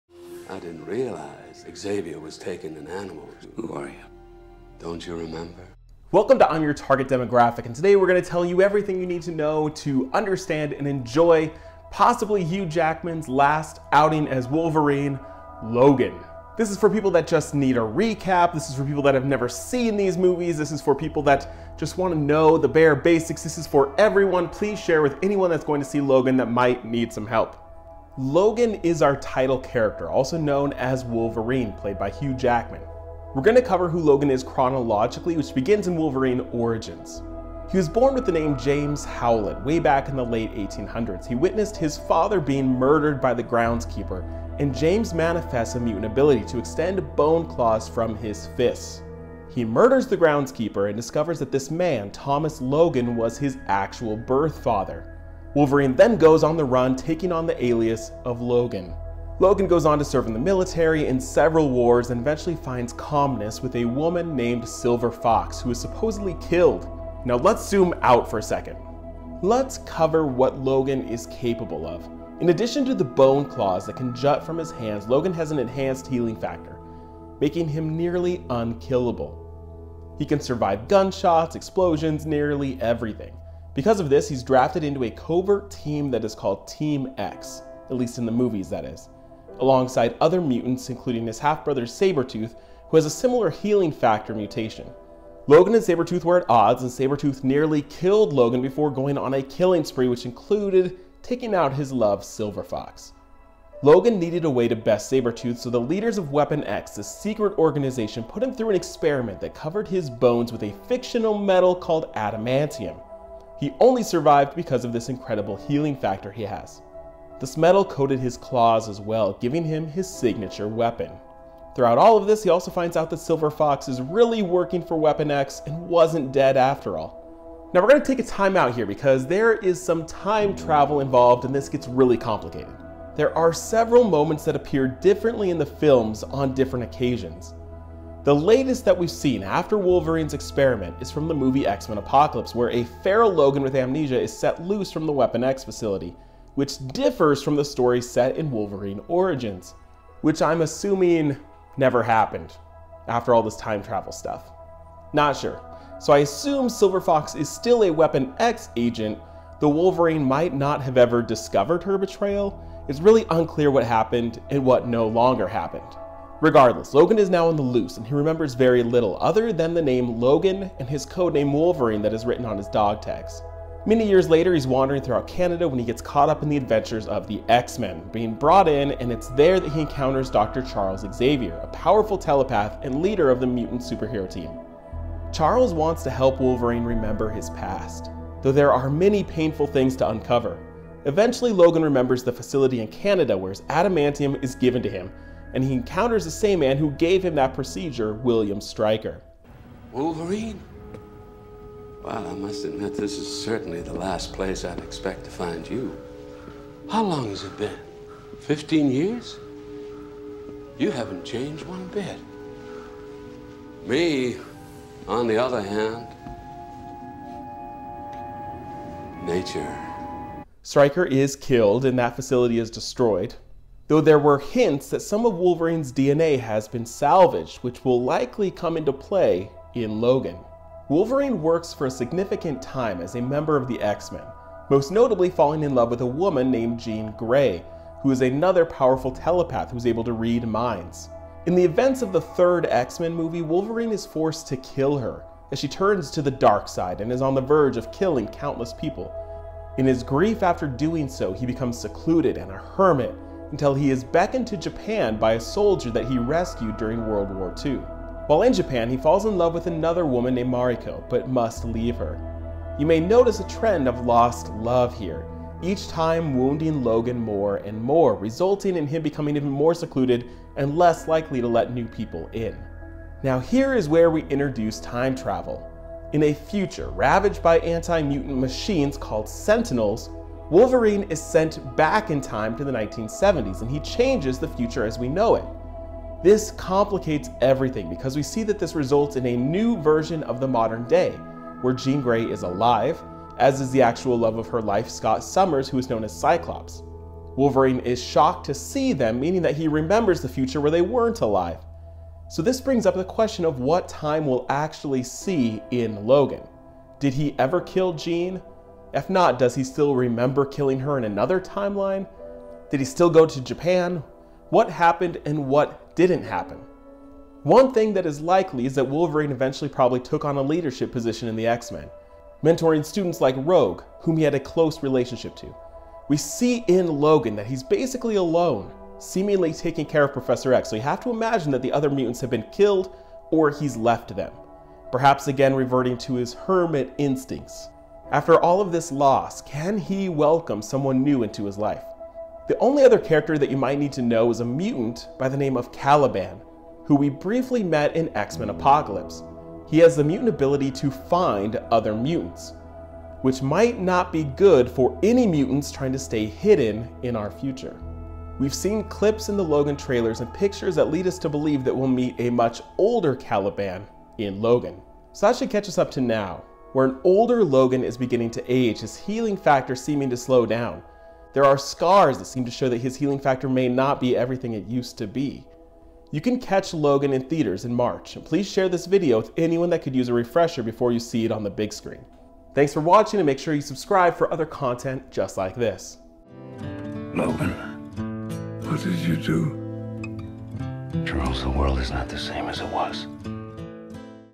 What You Need to Know: Logan
Everything you need to know before catching the next Wolverine movie, "Logan."